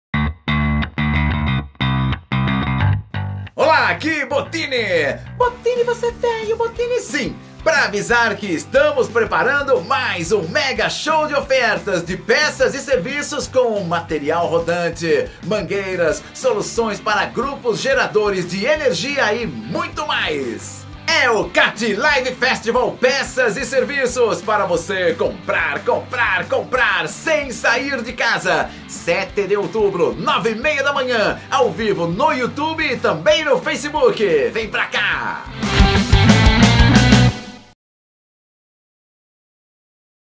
Cat Live Festival - Prepare-se para mais uma edição!
Estamos trazendo mais um show de ofertas e condições imperdíveis em peças e serviços, com apresentação de Ciro Bottini. Assista o vídeo e já reserve na sua agenda, o Cat Live Festival acontece no dia 7 de outubro, às 9h30 (horário de Brasília) e está cheio de novidades para você!